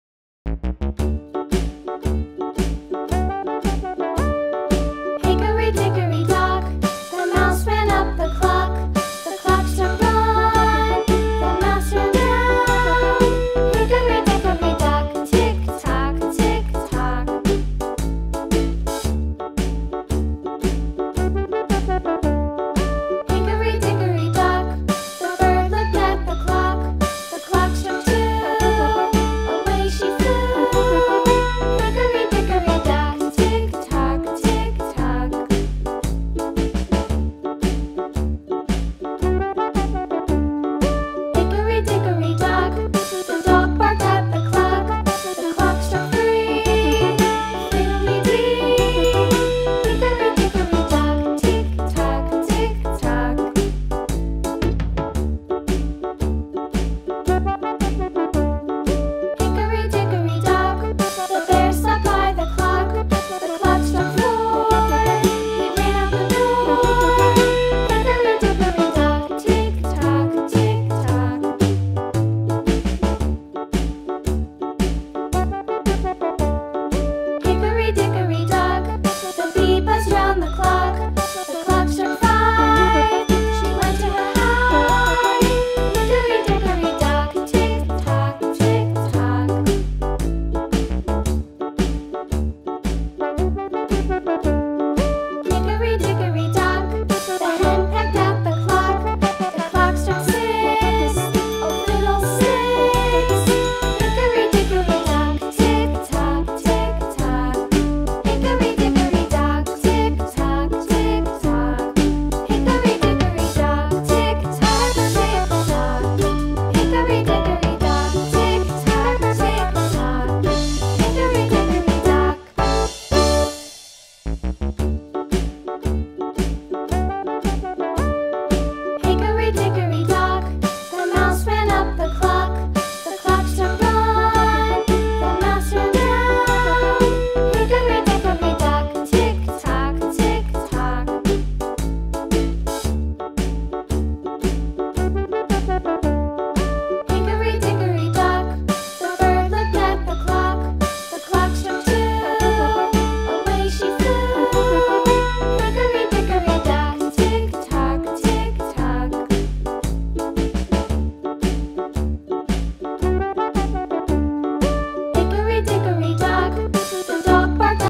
Hündchen Zeichnen und Malen für Kinder
Heute lernen wir wie man hündchen zeichnet!